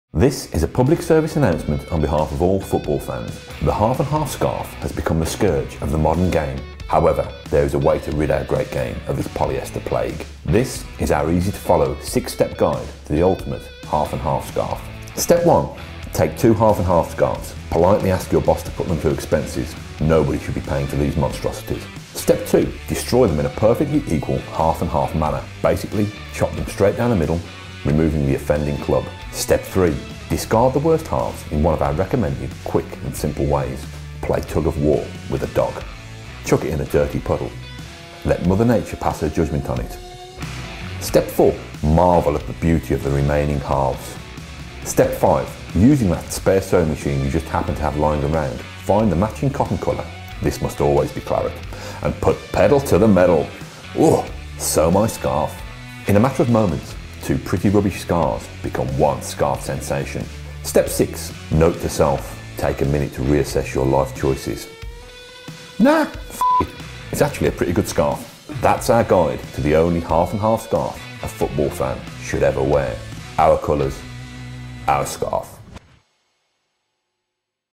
The Ultimate Half & Half Aston Villa Scarf
We can’t stand half and half football scarves. So we decided to create the only half and half scarf an Aston Villa fan should ever need...